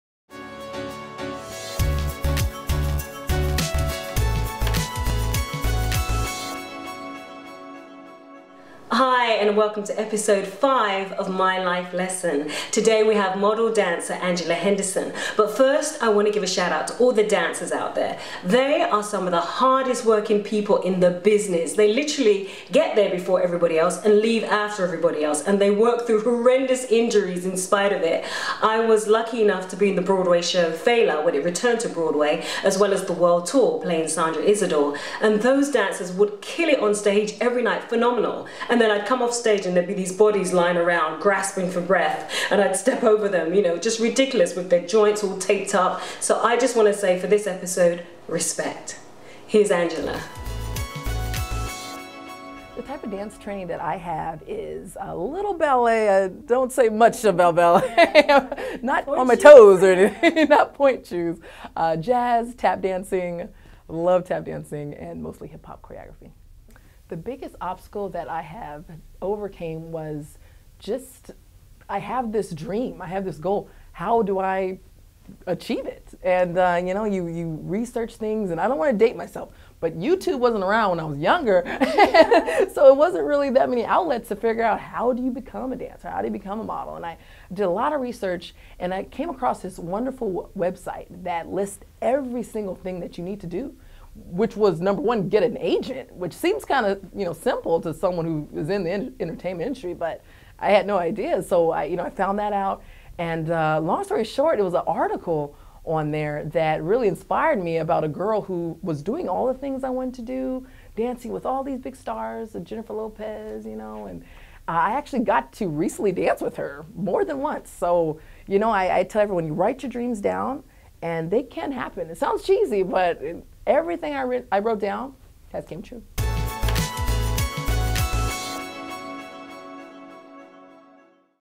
BEYONCE DANCER TELLS HER STORY
A resourceful Angela Henderson found the 'knowhow' she needed to launch her dance career.  More informative vids are posted on Answers4dancers.com